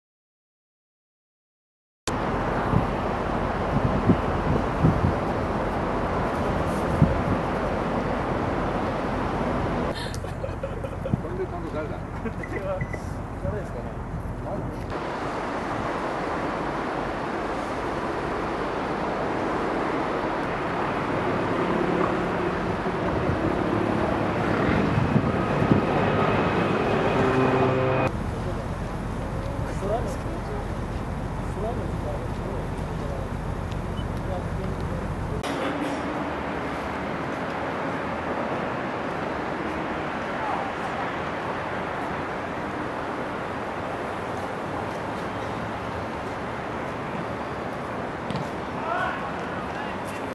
復興願う明かり 神戸ポートタワーとホテルオークラ神戸 東日本大震災
東日本大震災から１カ月となった１１日、神戸市内では、被災地の復興を願う明かりがともされた。ホテルオークラ神戸（同市中央区）が客室照明で建物東側に「ファイト」の文字を浮かべ、神戸ポートタワー（同）には発光ダイオード（ＬＥＤ）による「ガンバロウ！東日本」の文字が輝いた。